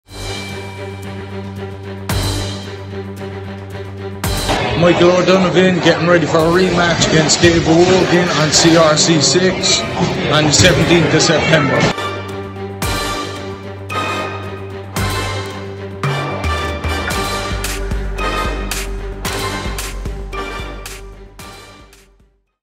CRC 6 - Pro K-1 63kg Title Fight Announcement Michael O'Donovan (Samson Martial Arts)
CRC 6 September 17th , Dublin  CRC is coming with an action-packed night of PRO and Amateur fights in incredible and unique Cage Ring  MMA / K-1 / Muay Thai / Submission Fighting !
It's going to be another great night , full of exciting fights !!
A few of really well-known, world level names and many tough local fighters will be in action!!
The show is fully covered by Fightbox TV
Event created 100% by people with a passion and total dedication to martial arts.